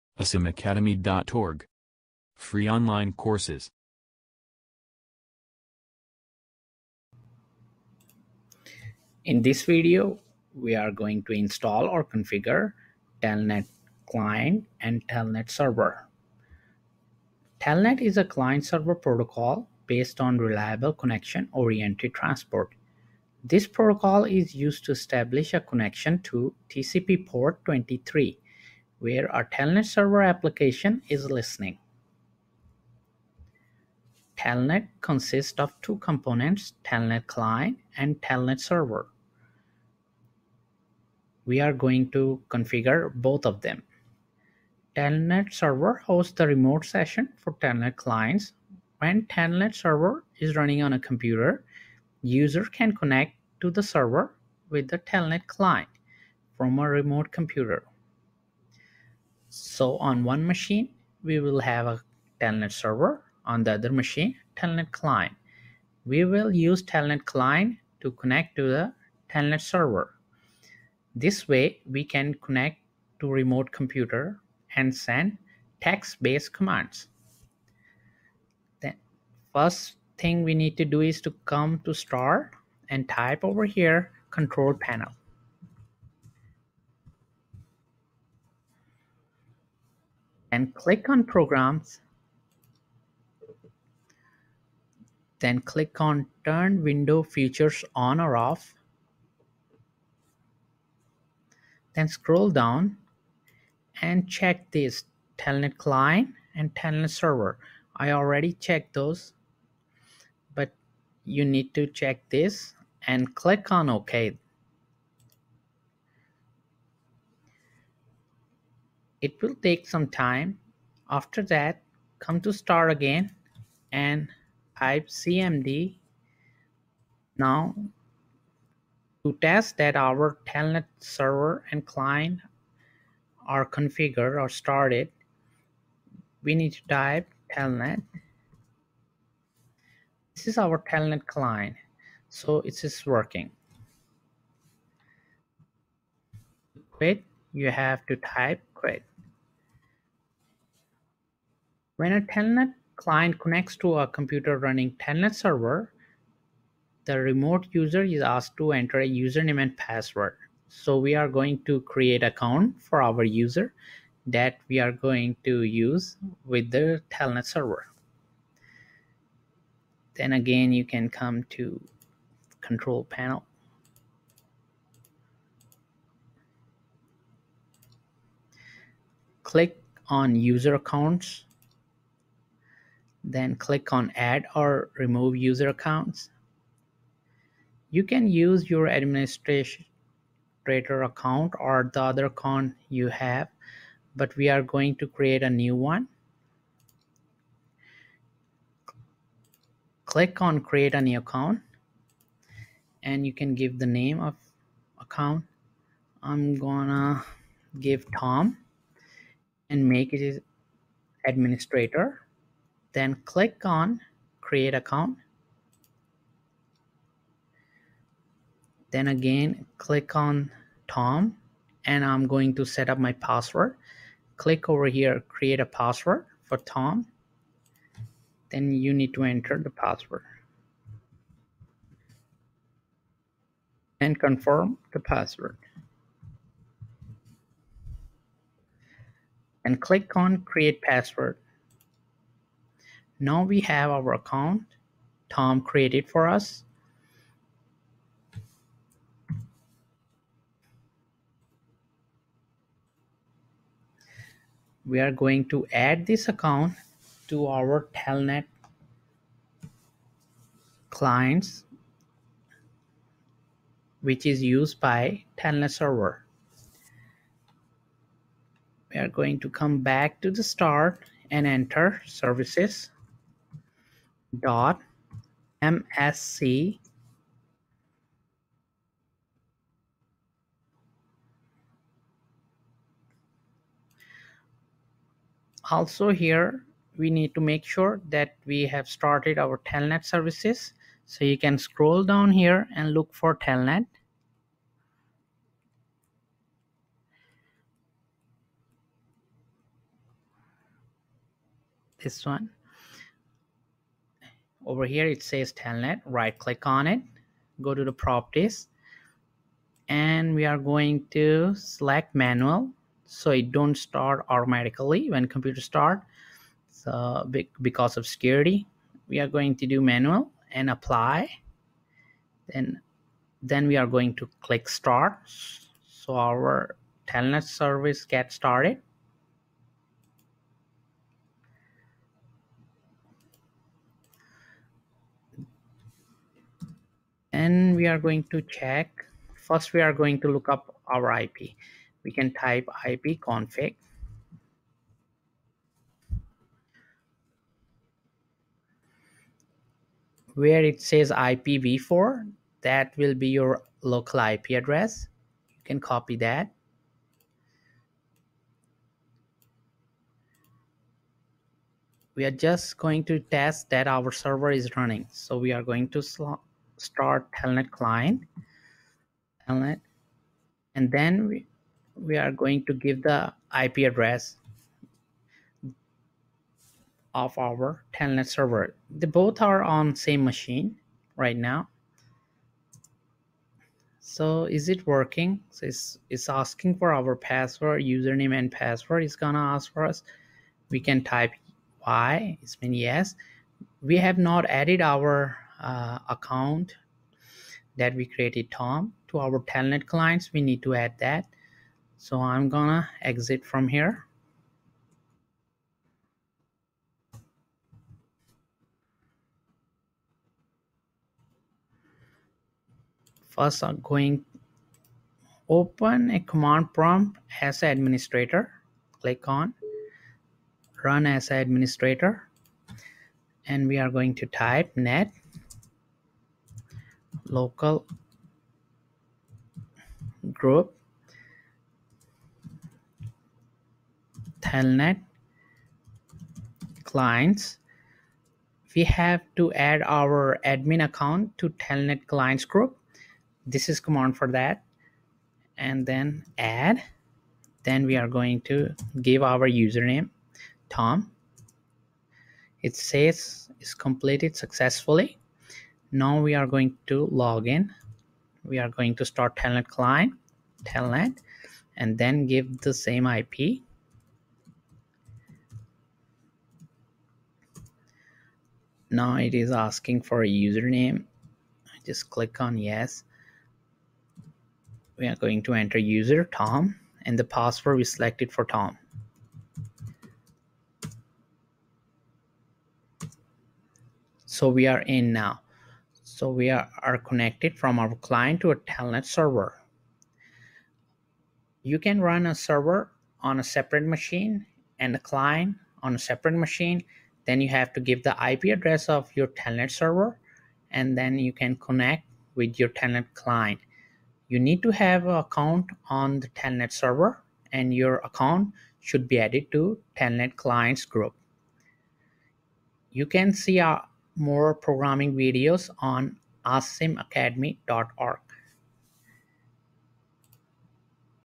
Installing Telnet Server And Telnet Client On Windows For Remote Computer Access By Asim Iqbal
Online Computer Courses